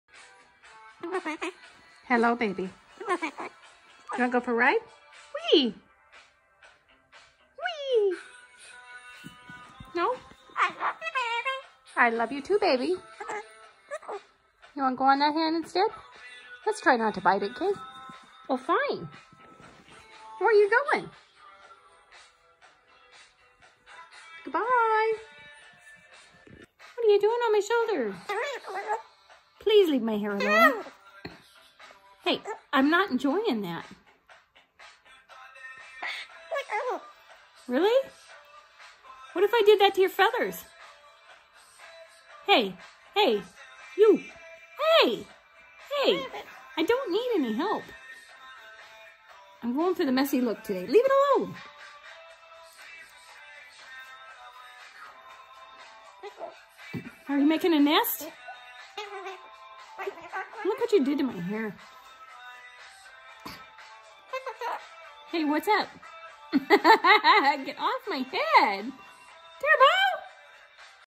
Cockatoo Turbo random clips from this afternoon 😬
Just two little video shorts put together from this afternoon. ⭐️Turbo.